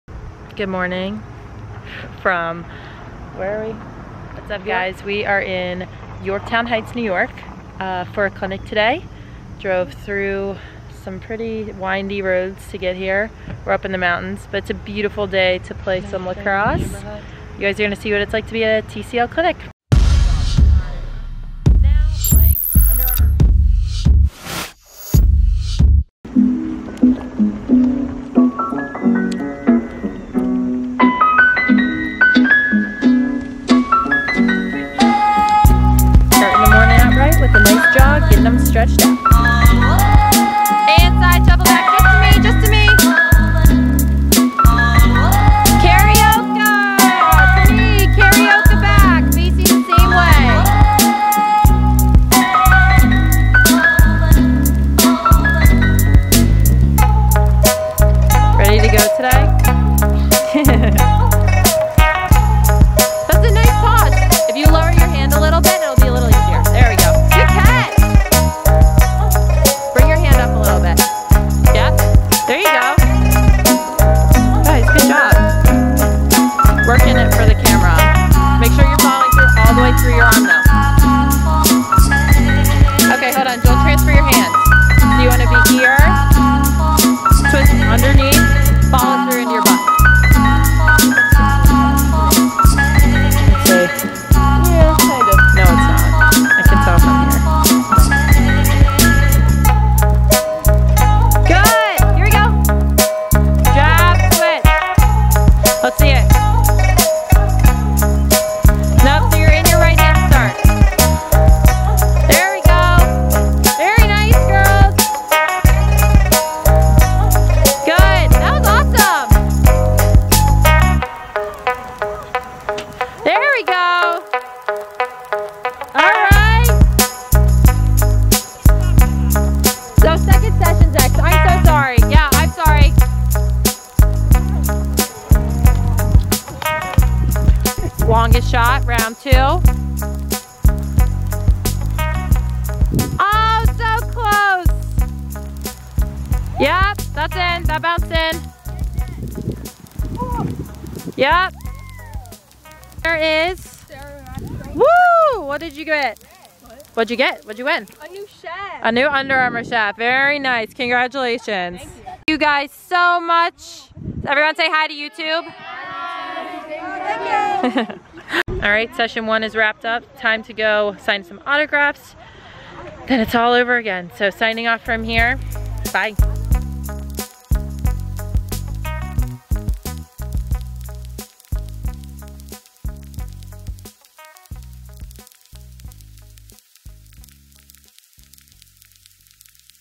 Back at it! | Clinic Time
TCL is back on the road! Take an inside peak of a recent lacrosse clinic.

Follow me on the social medias...